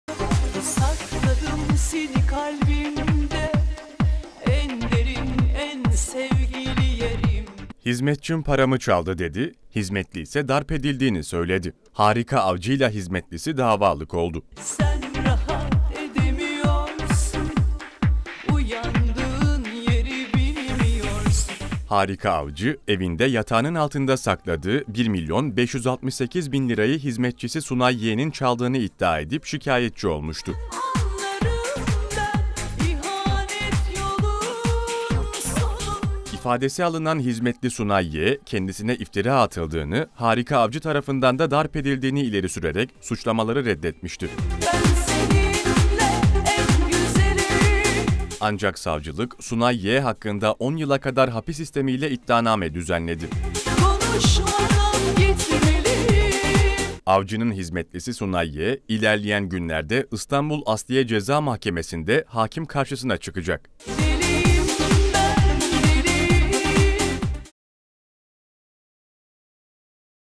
Harika Avcı'ya hırsız şoku! Yatağın altında 1 milyon 568 bin lira...
Ünlü şarkıcı Harika Avcı evinde sakladığı 1 milyon 568 bin liranın çalındığını ifade ederek yanında çalışan kişiden şikayetçi oldu. Kendisine iftira atıldığını savunan hizmetli Sunay Y. hakkında 10 yıla kadar hapis istemiyle iddianame düzenlendi.

Ünlü şarkıcı Harika Avcı, geçtiğimiz yıllarda hizmetlisinden şikayetçi olmuştu. Avcı, ifadesinde yatak odasında bulunan bazanın altında sakladığı paranın hizmetlisi Sunay Y., tarafından çalınmış olabileceğini çünkü, söz konusu paranın bulunduğu yere sadece hizmetlisinin girebildiğini ileri sürmüştü.

Avcı'nın şikayeti üzerine başlatılan soruşturmada ifadesi alınan Sunay Y., parayı çalmadığını, kendisine iftira atıldığını, aslında Avcı tarafından kendisinin darp edildiğini ileri sürerek suçlamaları reddetmişti.

Savcılık yürütülen soruşturma sonunda Sunay Y. hakkında 'Bina İçinde Muhafaza Altına Alınmış Olan Eşya Hakkında Hırsızlık' suçu kapsamında 10 yıla kadar hapis istemiyle iddianame düzenledi. İddianamede olay tarihinde bazanın altında bulunan 1 milyon 568 bin liranın hizmetliden başka kimse tarafından bilinmediği, paranın kendisine sorulduğunda ise saldırgan davranışlarda bulunduğu anlatıldı. Sunay Y., ilerleyen günlerde İstanbul Asliye Ceza Mahkemesi'nde hakim karşısına çıkacak.

tv100 uydu bilgileri:

TÜRKSAT 4A
Frekans: 12729
Sembol: 30000
Polarizasyon: V (Dikey)